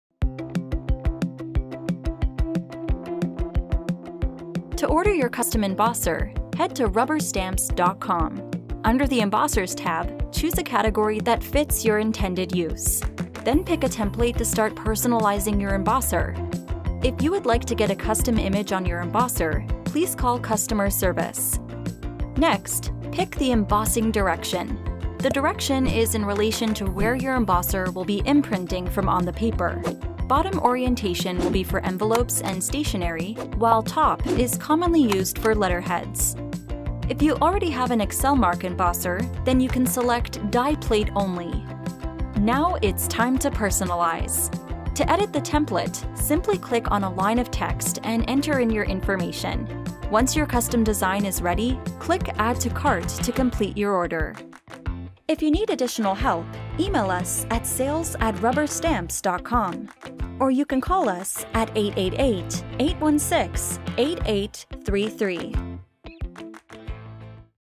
How to Customize Embossers
Embossers create a raised surface on paper and are perfect for envelopes, cards and letterheads.